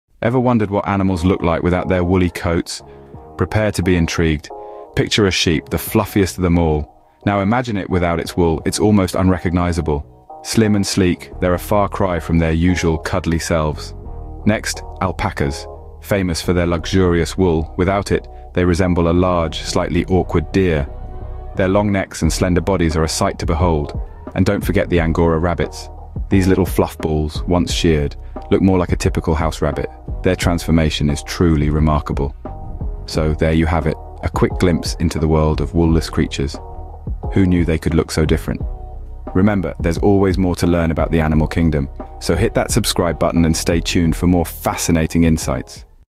What do animals look like without wool?
In this fascinating video, we dive into the world of animals without their wool, revealing their surprising appearances. From sheep to alpacas, get ready to witness the incredible transformations these creatures undergo once their wool is removed.

Join us as we explore the unique features and characteristics of these animals, showcasing their true beauty underneath all that wool. Discover how different species adapt to different environments, and gain a new appreciation for their stunning natural forms.

With stunning visuals and insightful information, this video is a must-watch for animal lovers and nature enthusiasts alike.

OUTLINE:

00:00:00 Unveiling Animals: Beyond the Wool